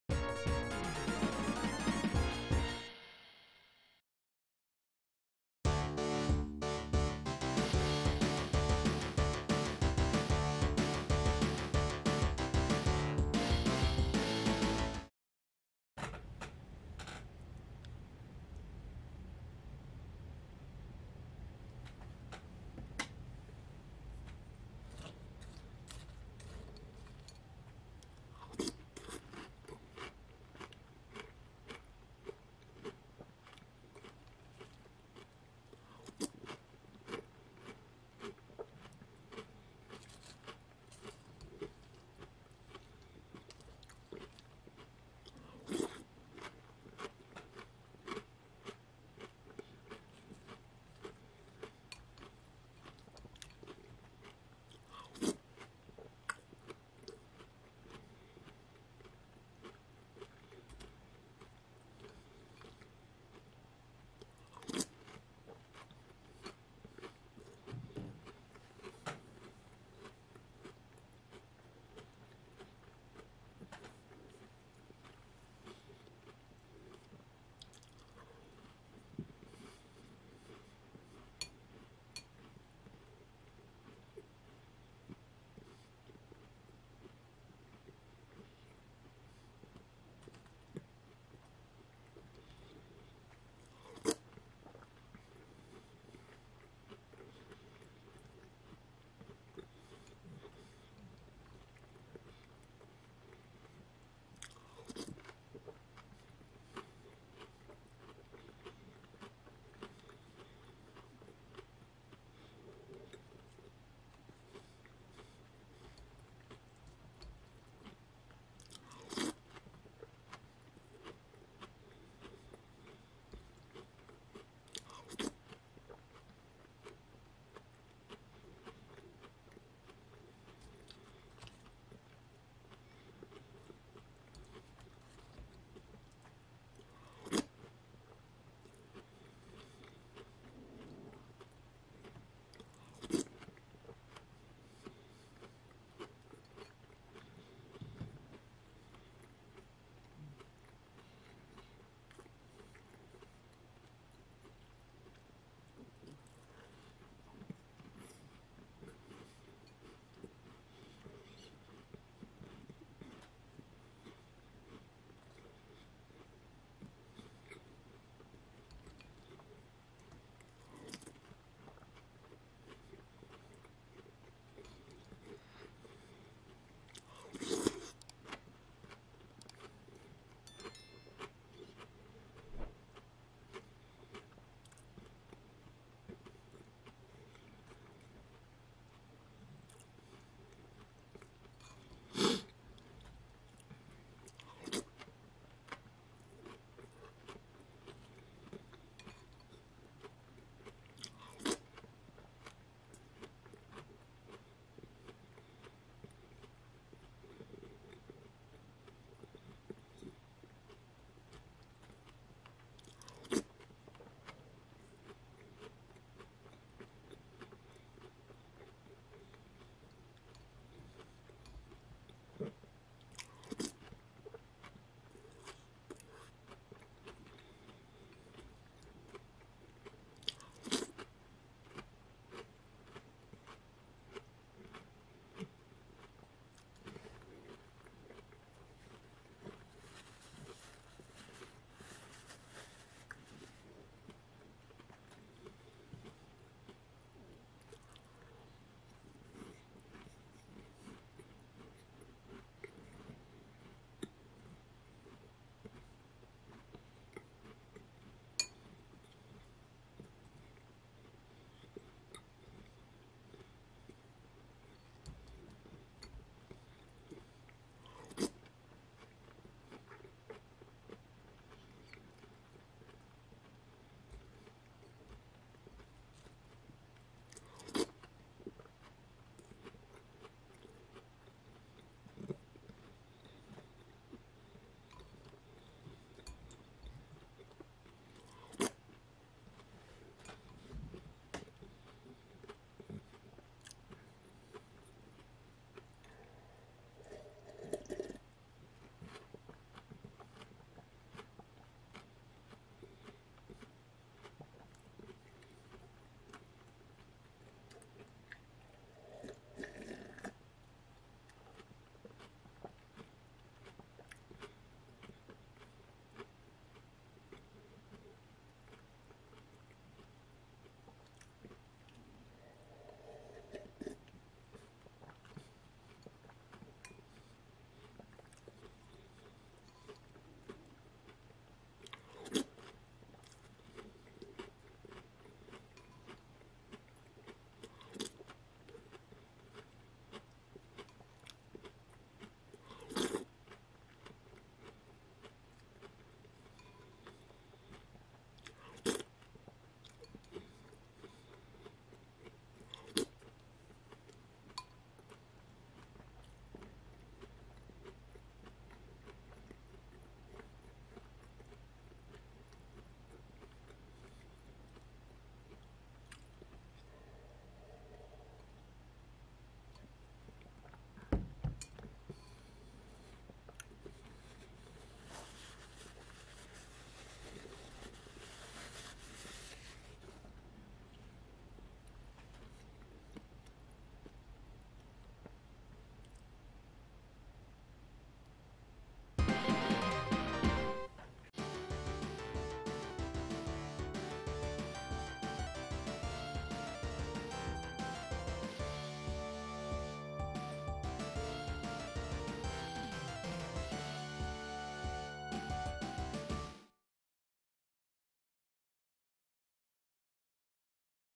CINNAMON TOAST CRUNCH & COCONUT RASPBERRY COOKIES ASMR MUKBANG
Today we have a bowl of Cinnamon Toast Crunch & a few Coconut Raspberry Cookies!